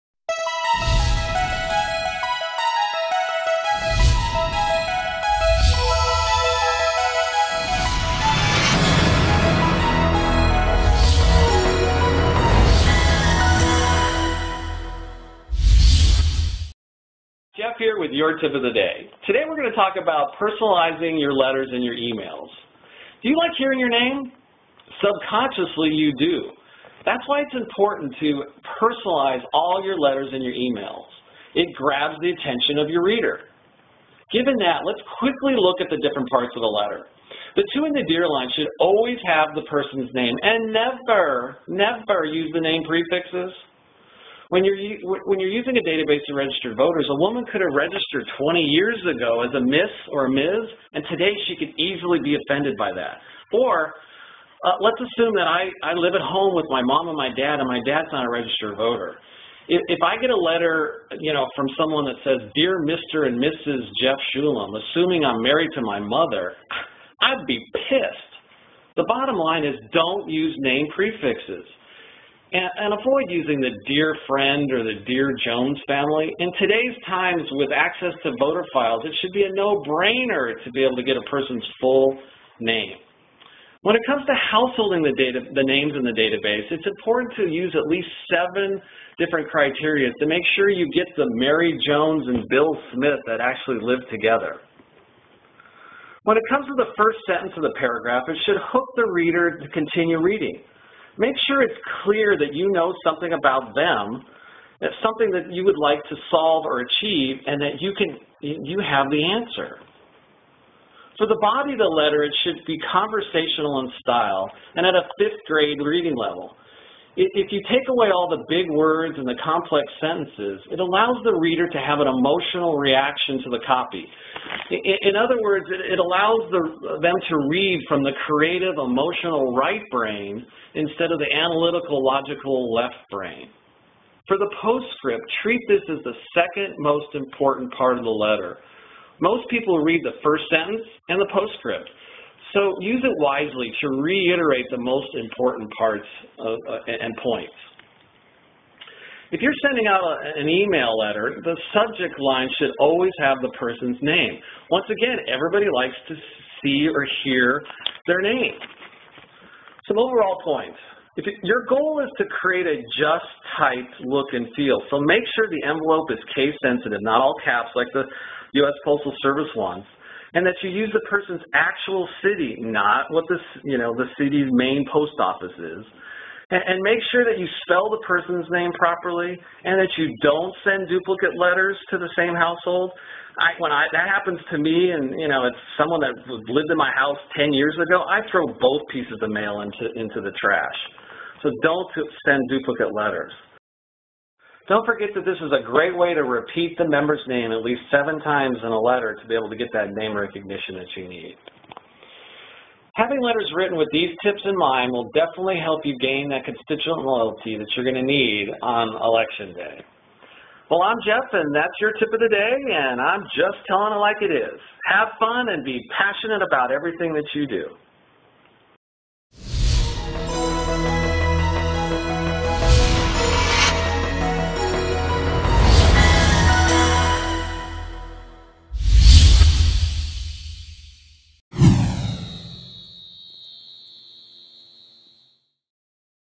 Tip of the Day - Personalized Letters for Political Mailings
Stock / Form letters are OUT and Personalized Letters are IN. Here are some tips to getting the constituent loyalty you deserve by personalizing the targeted political mailings you do.